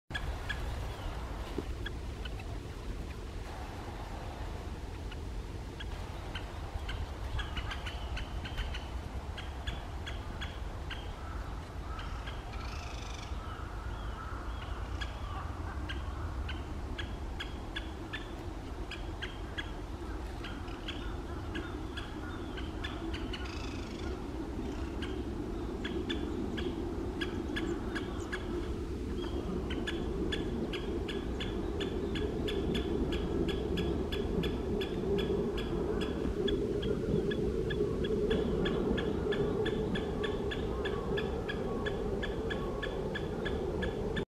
アカゲラ 追いかけっこ 繁殖期 2023.4 [北海道の野鳥]
@旭川市　メスを追うオス　カップルになれたかな